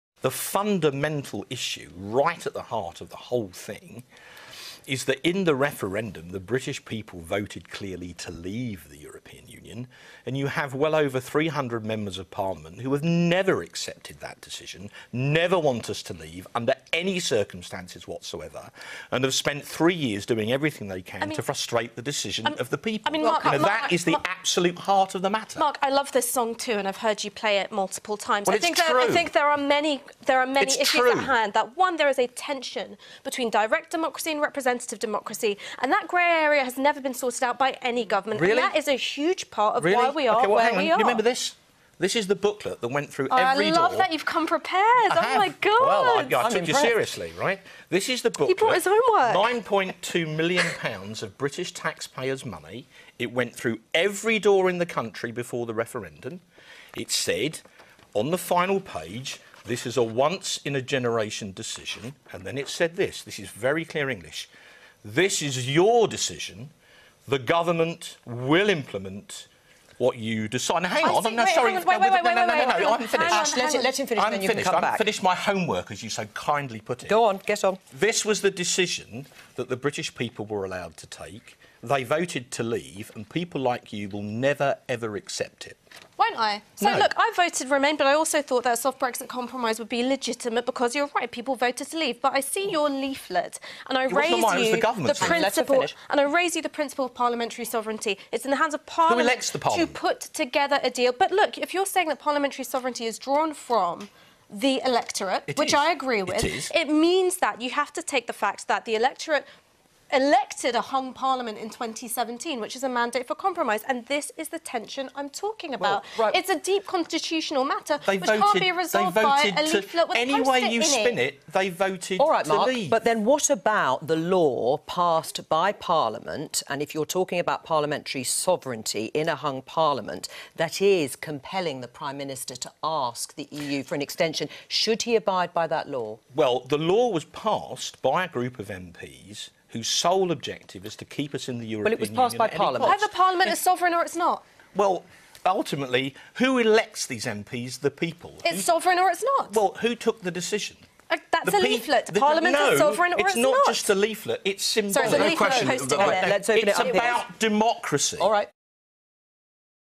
The 2016 referendum wasn’t a mandate for no deal and hard Brexit
Ash Sarkar, intelligently and calmly, skewers Mark Francois argument about the mandate of the 2016 referendum. Brexit wasn’t a mandate for no deal and hard Brexit - end of story!